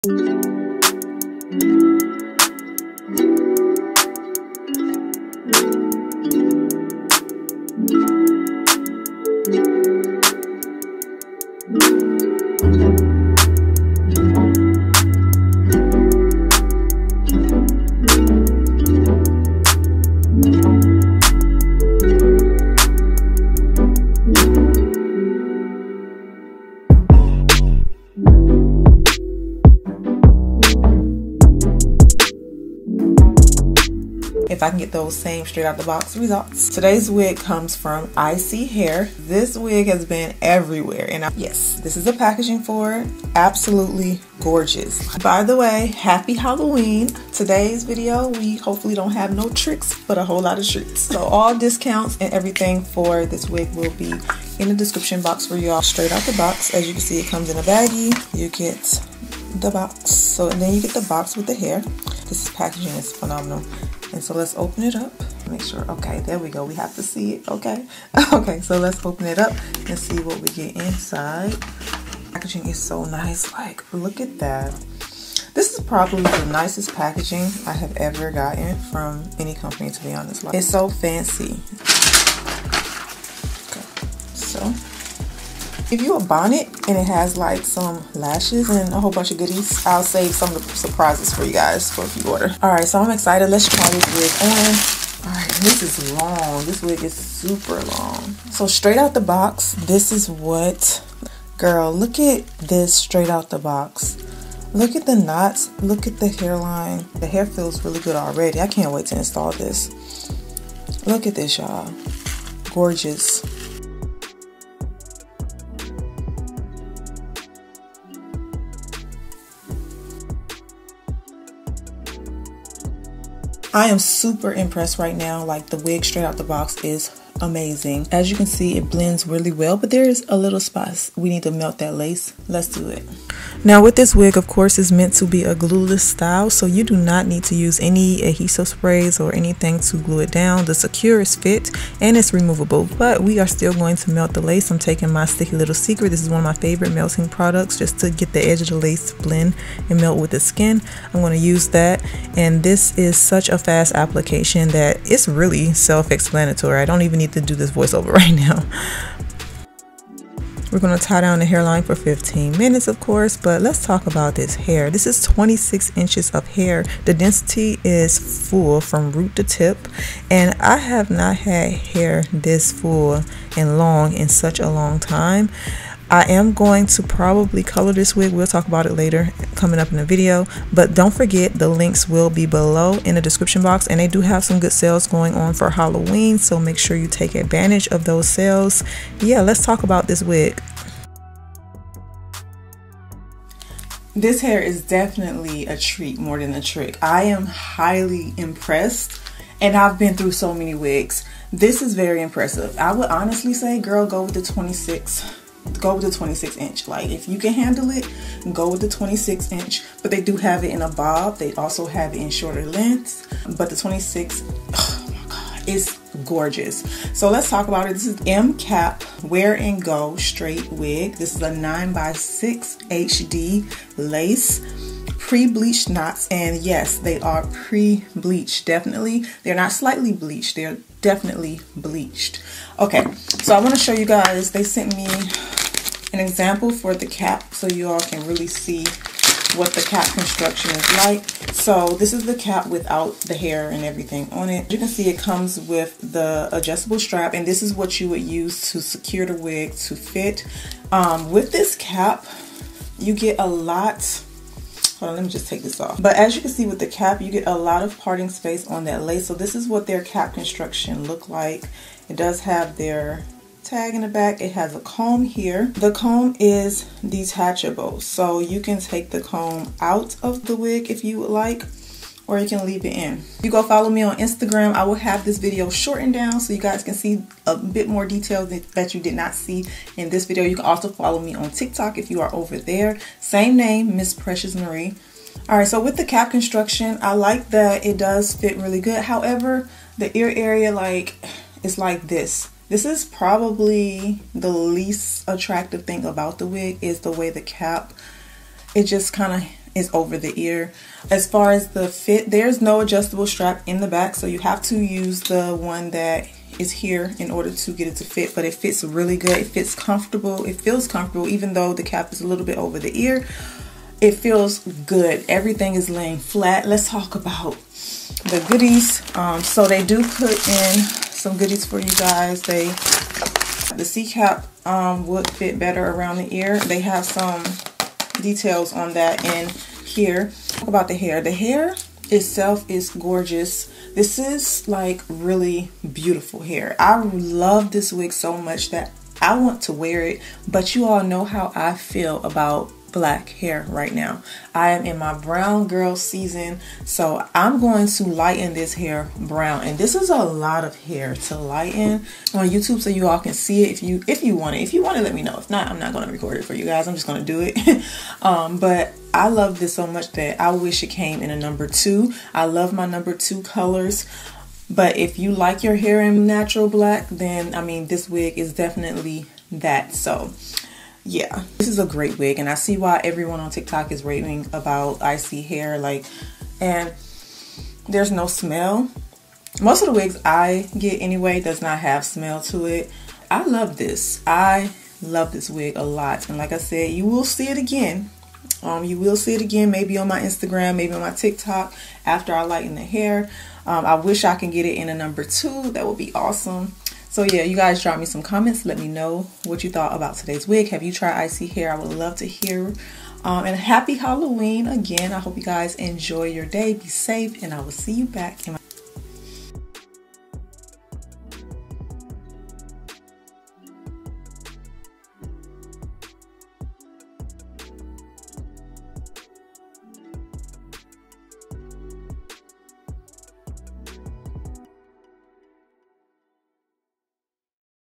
ISEEHAIR...🎃 Trick Or Treat: 26 INCH M- Cap PRE-CUT Wear Go Wig (BEST SELLER WIG)
Cap Design: ISEE M-CAP 9*6 Lace Size
Length: 26 inch
Edges: Pre-plucked
Lace: Pre-cut
Knots: Pre bleached tiny knots

▬▬ ISEE M-Cap Wear Go Wig ▬▬
▶Maximum Lace Size
▶Minimal Knots
▶Match Ear Shape
▶Movable Combs
▶Mimic Forehead Contour
▶Mark Of Authenticity

Every product and tool I use will be in this link.

TIMESTAMPS:

*Hair or products shared are my OWN personal and honest experience. Thank you for being so supportive!*